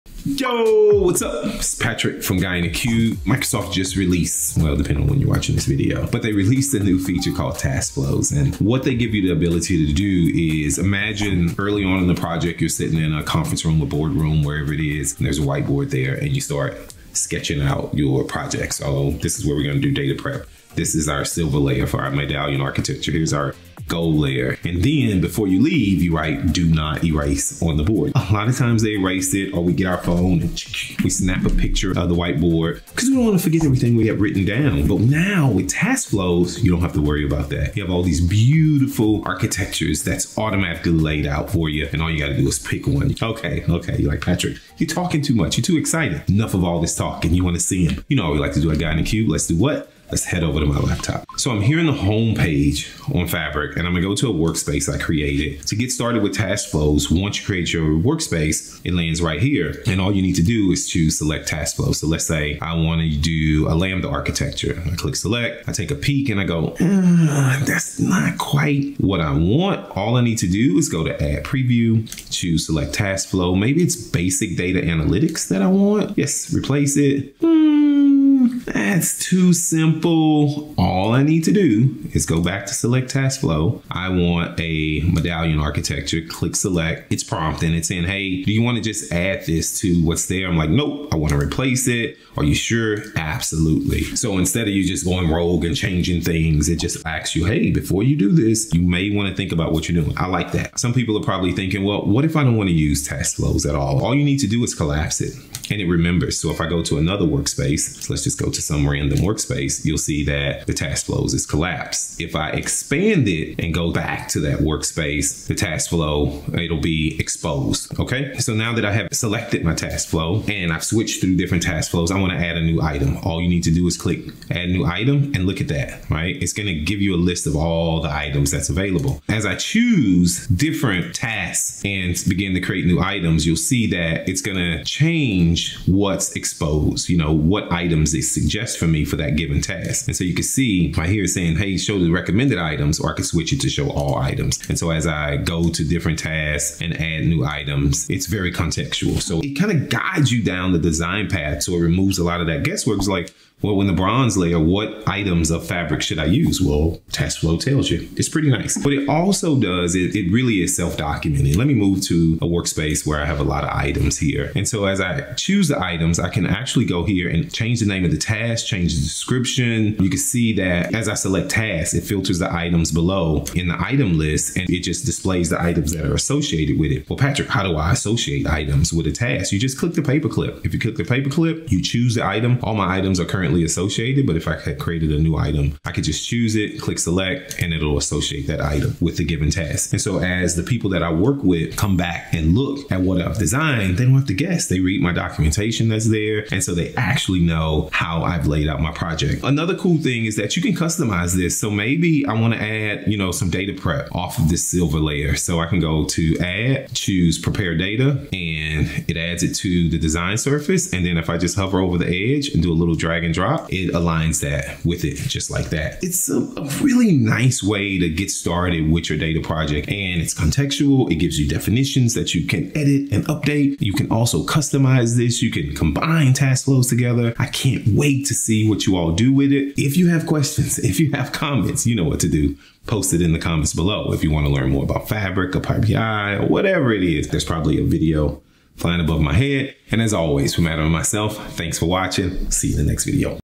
Ditch the whiteboard! Use Microsoft Fabric task flows!
Tired of drawing out your flow on a whiteboard and it getting erased? Now you can use task flows within Microsoft Fabric to lay things out. Patrick gets you started!

Announcing the public preview of task flows in Microsoft Fabric